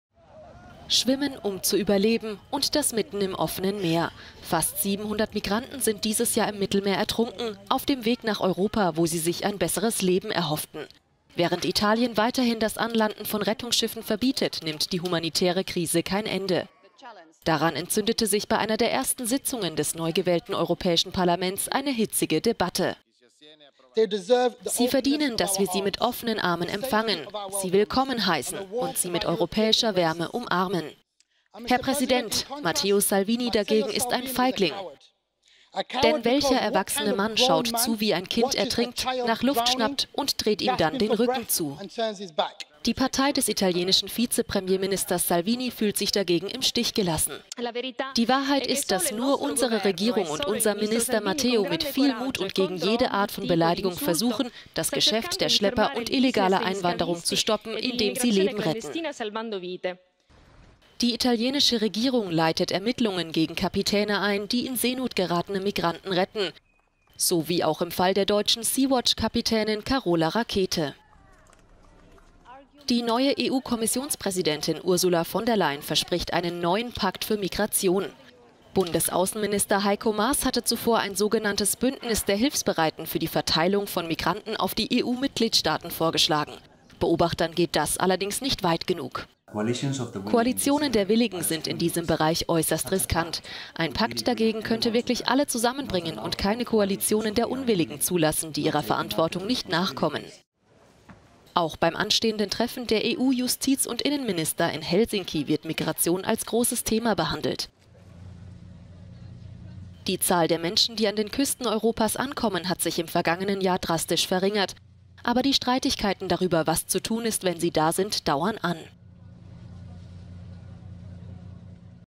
Neugewähltes Europaparlament: Hitzige Debatte um Migration
Fast 700 Migranten sind dieses Jahr auf dem Weg nach Europa im Mittelmeer ertrunken. Jetzt lieferten sich EU-Abgeordnete einen Schlagabtausch zur Migration im Parlament. …